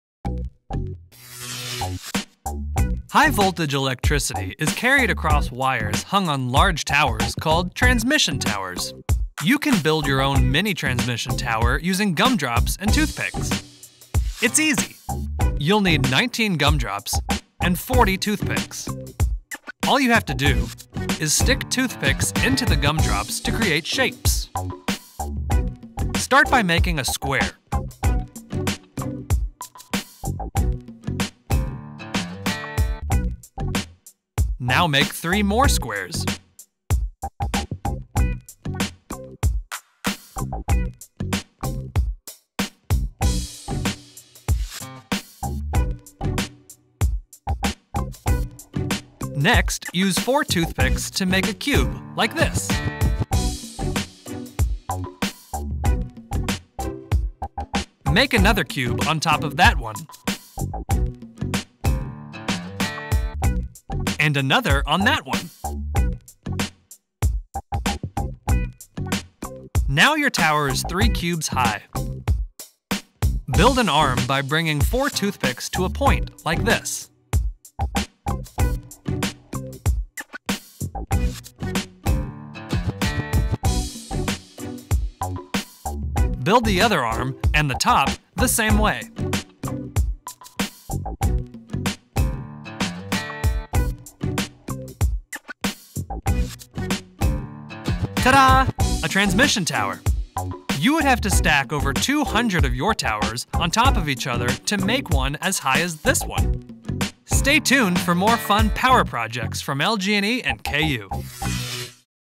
How to Build a Gumdrop Tower
Science, technology, engineering and math are at the root of what we do as an energy company. Watch this short video to learn how to build your own fun – and delicious – tower using some basic civil engineering concepts, gumdrops and toothpicks.